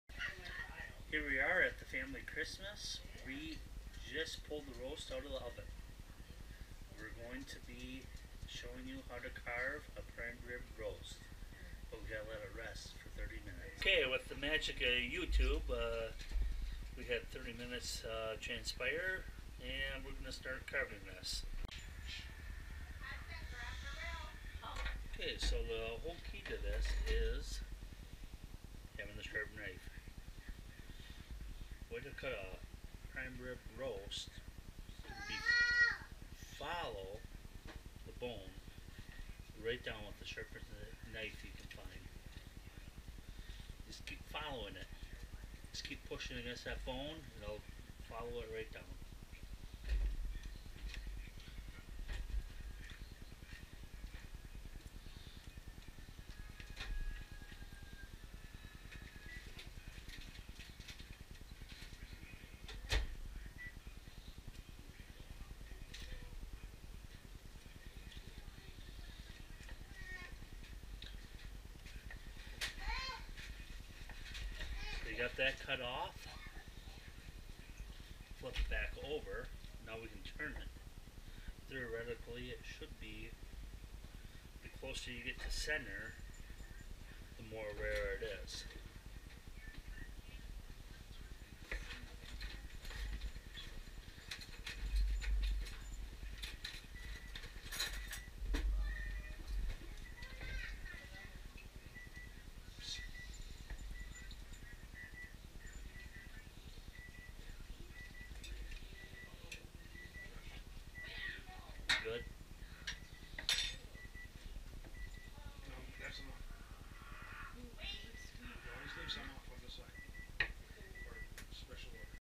How to carve prime rib gopro
Every year we make a Prime rib roast for our family Christmas.  By the time the roast is out of the over we are often toasted.  Any way it has been so good every year we have decided to make a video on how to carve it so we can remember every year :P If this video gets to 1000 views we decided we would share our family recipe!